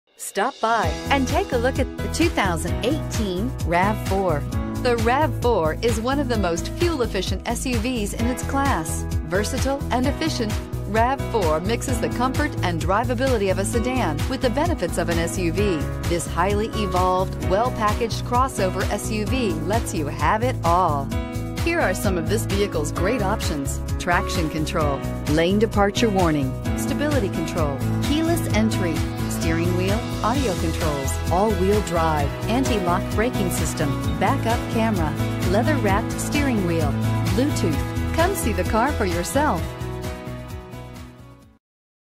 2018 Toyota RAV4 Lee's Summit MO 240857
2018 Toyota RAV4 XLE

Adams Toyota
501 NE Colbern Road

The 2018 Toyota RAV4 deliver more fun with a powerful 176hp 4-cylinder engine for off-the-line thrills. Coupled with the Dual Variable Valve Timing with intelligence technology, the RAV4 is a smart, spirited choice. More room means more space for gear and friends. You can quickly reconfigure the rear seats into a 60/40 split and fold the second-row seats down to create a wide, easily accessible load space with up to 73.4 cubic feet of capacity. RAV4 offers two available audio systems that incorporate navigations, so you can have the perfect soundtrack for all of your adventures. Travel safely with more peace of mind. RAV4 comes standard with a suite of six advanced safety features designed to help keep you out of harms way.